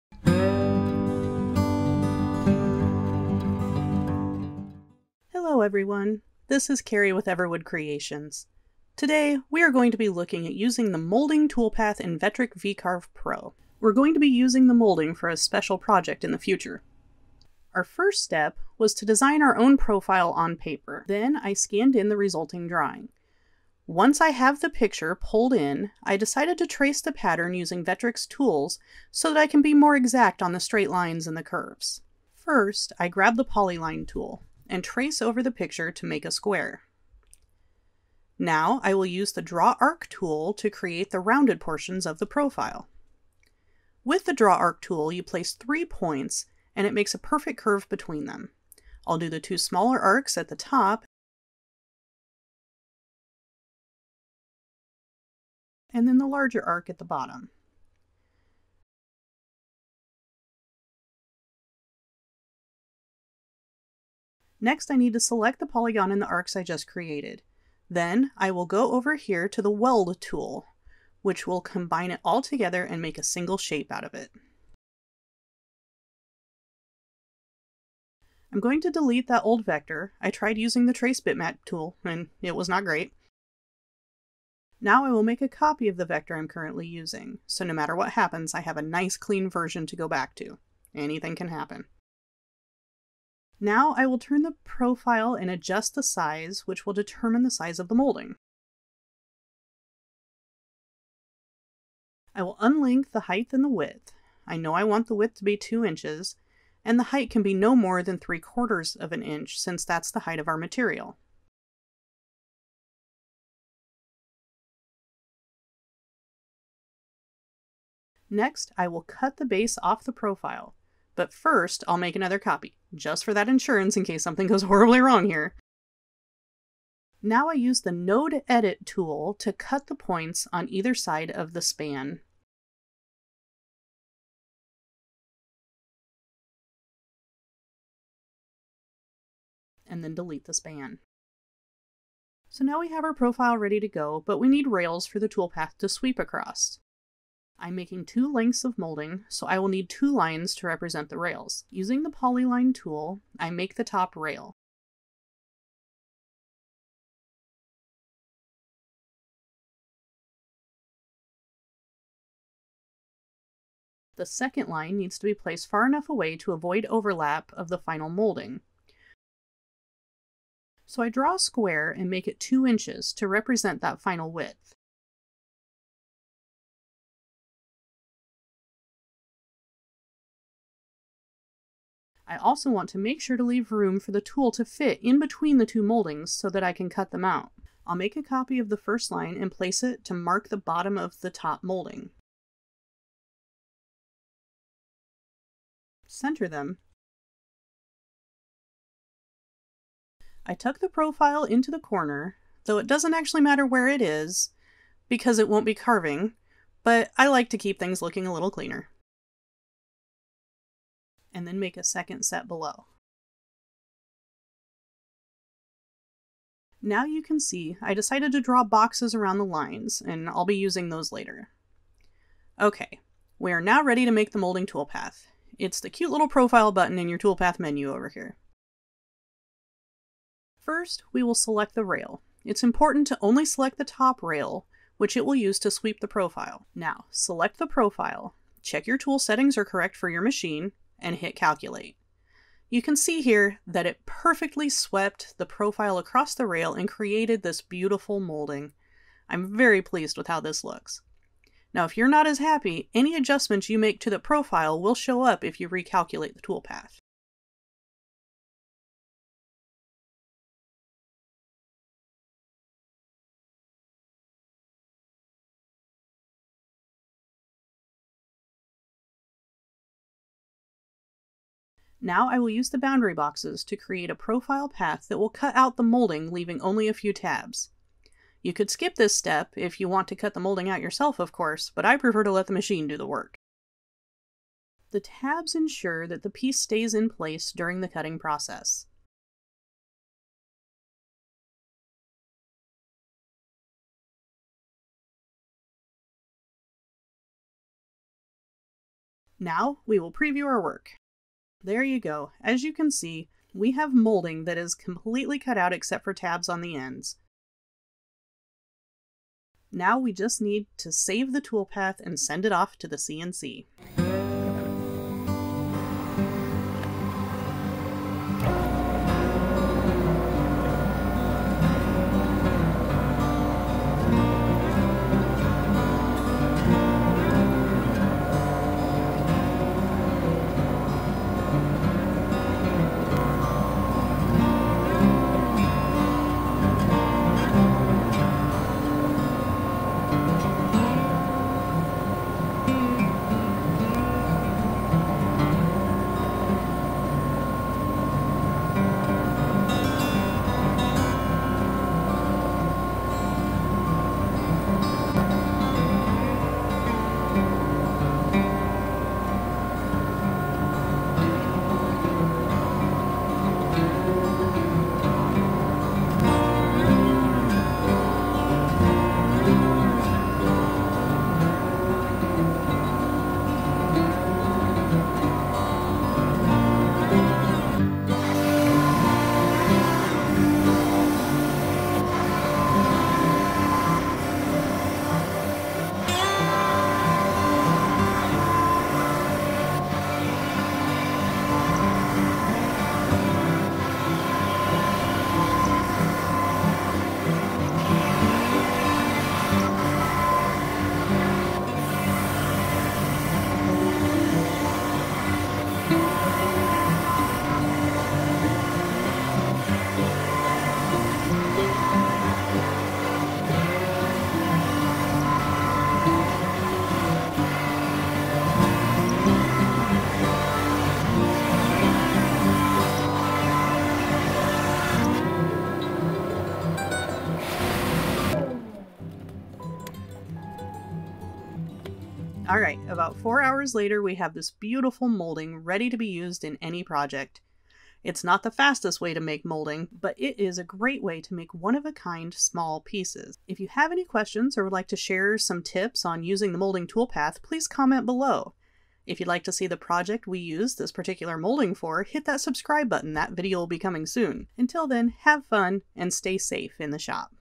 VCarve Pro Moulding Toolpath (Revised)
In this video, we take you through the steps of creating one-of-a-kind moulding using the Moulding Toolpath in Vectric VCarve Pro.

The CNC portions are at 10x speed with much cut out for the sake of viewing time, actual cut time for the moulding was about 4 hours.

Bits used include a 1/16" Ballnose for the moulding and 1/4" Endmill for the cutout.

*This video was revised from the original to take care of problems with the voiceover and audio quality